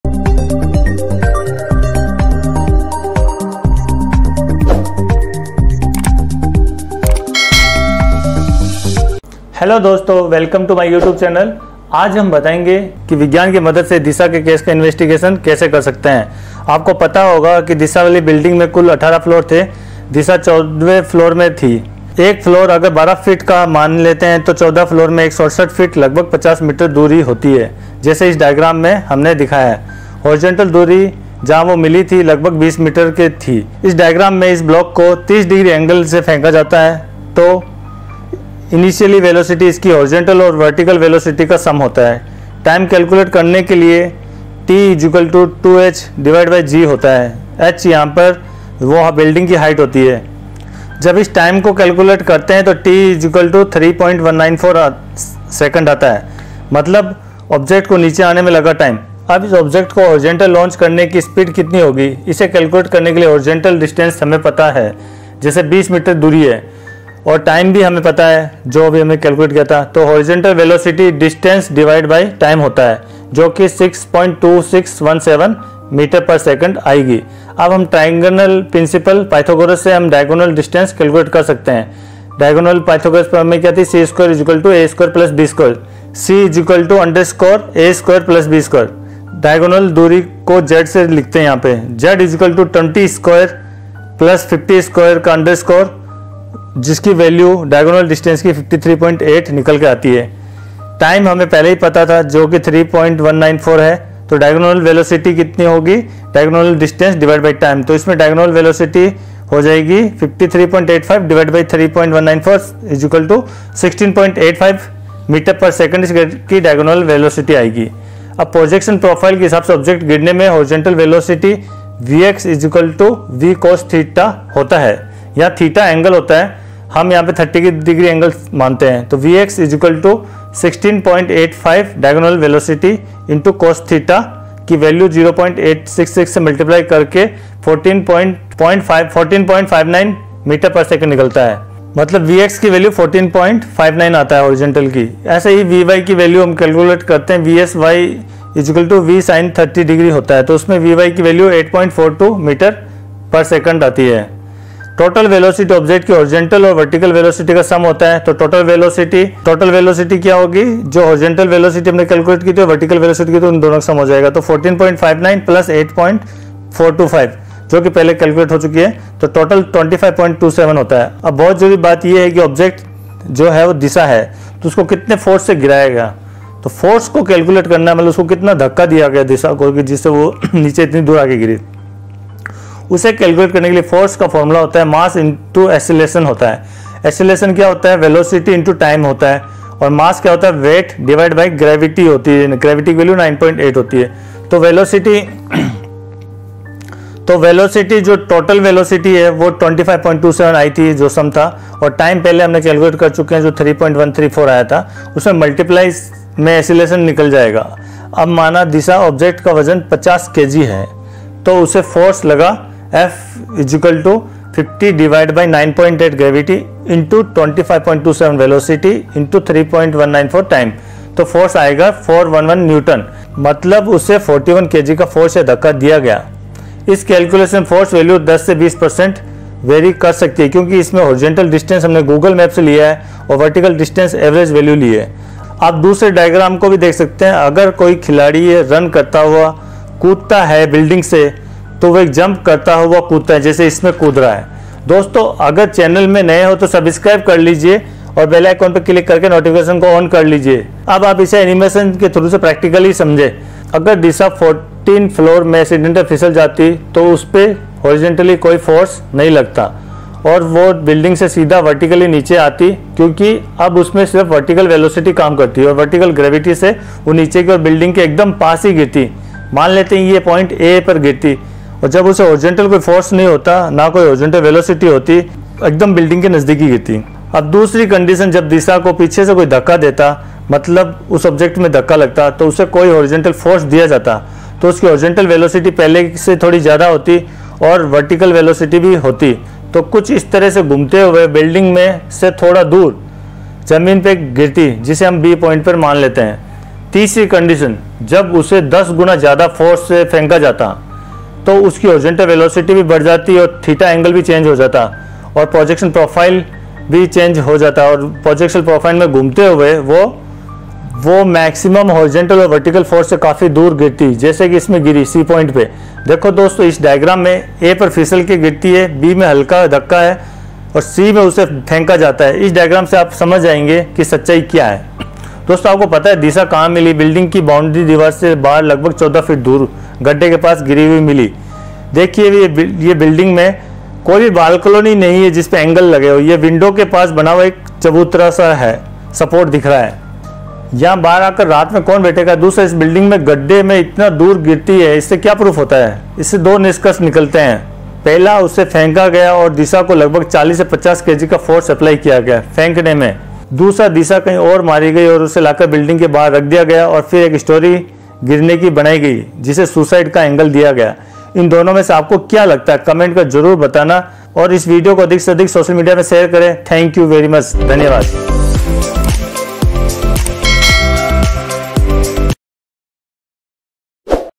Force Measure by Projectile Motion use of Physics in Disha Case Theory.
This video just for education purpose and data may be vary and depend on so many factor like wind direction and actual distance or time calculation. This video only give approximate idea.

S..H..A...R..E. &. S__U___B___S___C___R___I___B___E U___S

Thanks